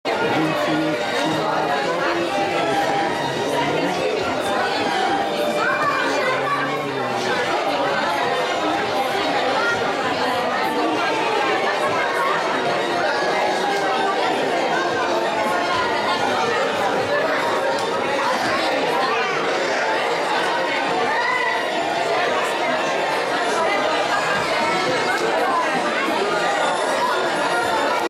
Mikulás Délután 2014.
A Csikágói Magyar Klub, és Cserkészek, rendezésével és Szervezetek, Iskolák résztvevésével